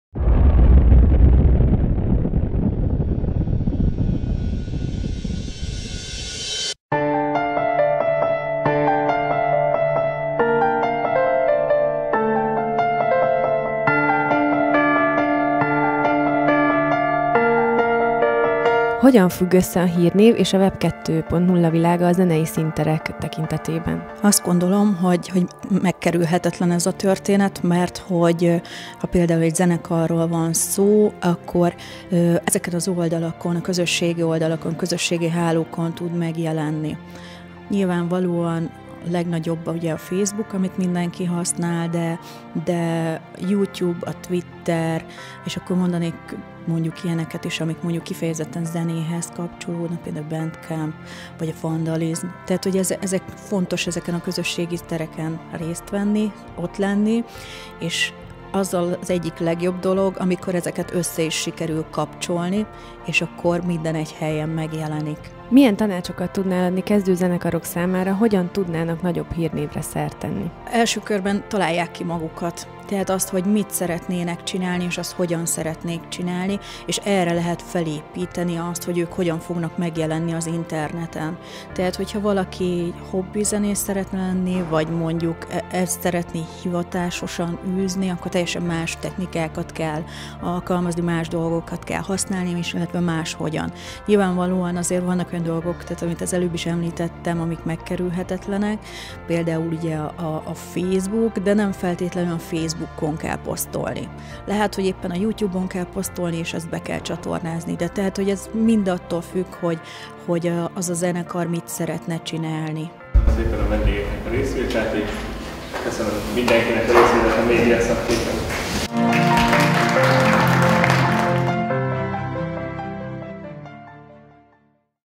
Média Szakhét #03 - Zenei színterek, hírnév és a web 2.0
A Média Szakhét zárásaként Zenei színterek, hírnév és a web 2.0 címen rendezett kerekasztal-beszélgetést a kommunikáció szak harmadik évfolyama.

A beszélgetés résztvevői:

Szabó Krisztina - DebrecenPlus főszerkesztője
Csende Gergely - kommunikáció- és médiatudomány szakos harmadéves hallgató
Damu Richárd - sub.hu munkatársa
Mészáros Péter - kommunikáció- és médiatudomány szakos PhD-hallgató

Moderátor:
Varga Máté, kommunikáció- és médiatudomány szakos harmadéves hallgató